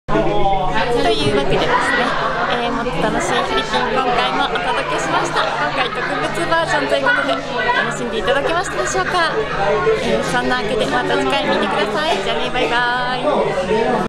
もっと楽しいフィリピン番外編、終了後
えー、締めコメせっかく撮ったんでマブハイ終了後に繋ぐはずだったんです！が、うっかり反転させて撮ってしまったため繋げない（泣）
　そんな間抜けな私の締めコメも笑いつつ覗いてやってくださいませ☆